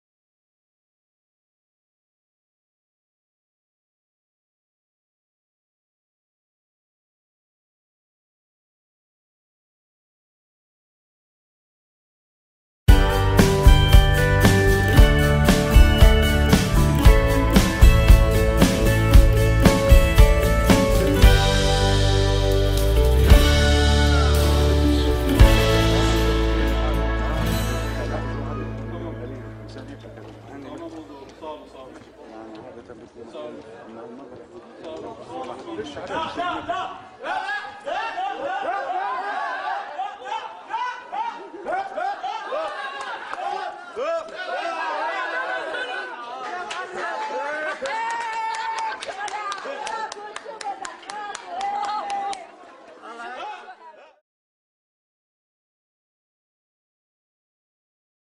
Israeli army Coward
Two Israeli IDF coward soldiers scared away of young brave armless Palestinians just shouting.